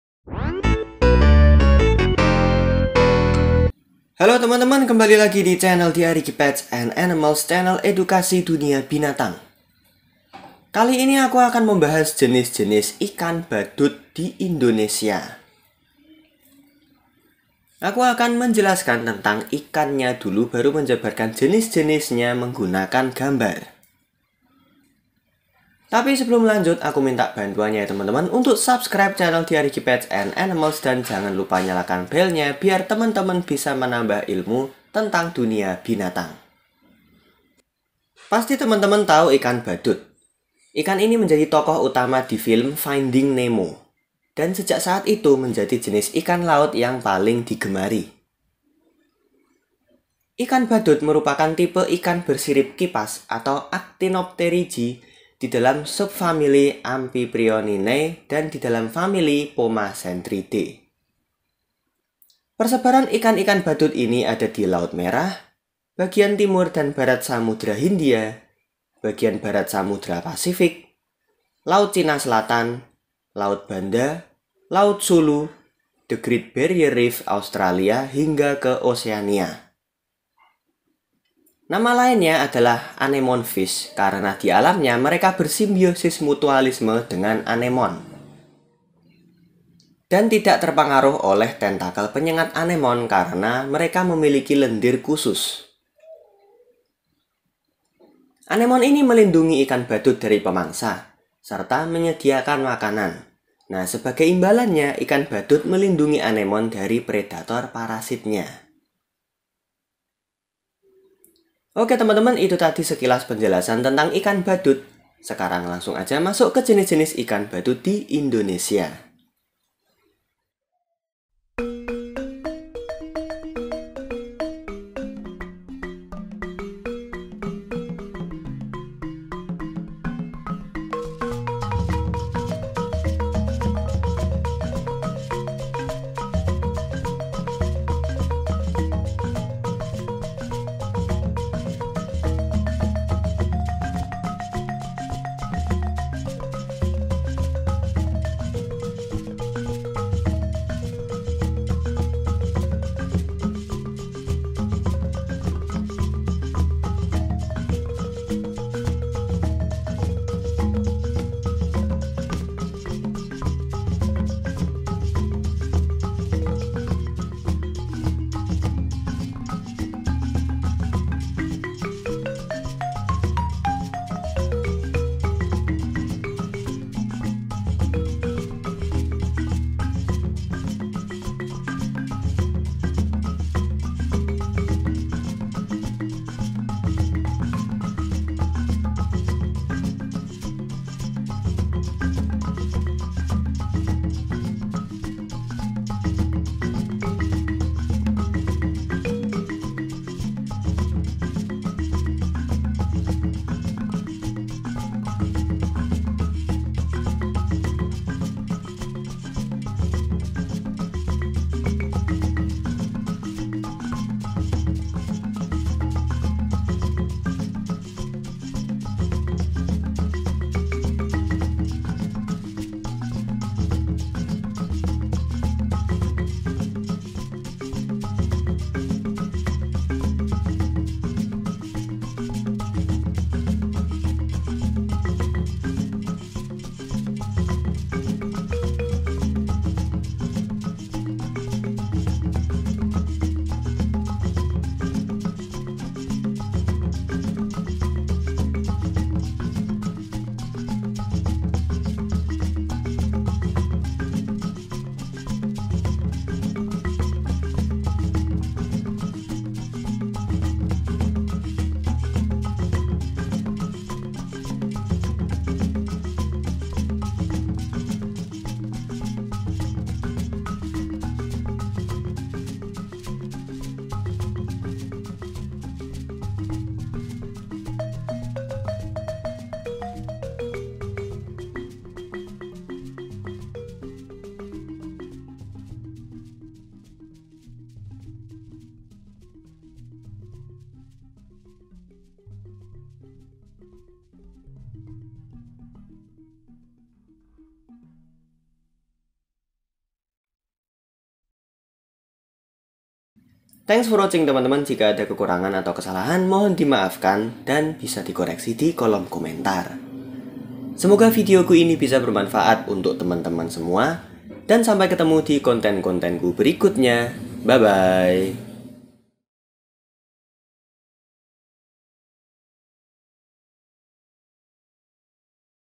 14 Jenis Ikan Badut (Clownfish / Anemonefish) di Indonesia
Video saya awali dengan penjabaran singkat seputar ikan badut di Indonesia, setelah itu baru menyebutkan apa saja jenisnya

Apa aja sih jenisnya di Indonesia?
- Common clownfish / Ikan giru ocellaris / Ikan nemo / Amphiprion ocellaris
- Percula clownfish / Ikan giru biak / Nemo biak / Amphiprion percula
- Maroon clownfish / Ikan balong / Amphiprion biaculeatus
- Clark's anemonefish / Ikan polimas kuning / Amphiprion clarkii
- Yellowtail clownfish / Ikan polimas hitam / Amphiprion sebae
- Orangefin anemonefish / Ikan giru pasir / Amphiprion chrysopterus
- Saddleback anemonefish / Amphiprion polymnus
- Tomato clownfish / Ikan tompel padang / Amphiprion frenatus
- Dusky anemonefish / Ikan tompel lombok / Amphiprion melanopus
- Skunk anemonefish / Ikan giru pelet / Amphiprion perideraion
- Orange skunk clownfish / Amphiprion sandaracinos
- Yellowbelly skunk clownfish / Amphiprion akallopisos
- White bonnet anemonefish / Amphiprion leucokranos
- Thielle's anemonefish / Amphiprion thiellei

Foto hewan di thumbnail oleh Jeffrey T. Williams dan orafarm.com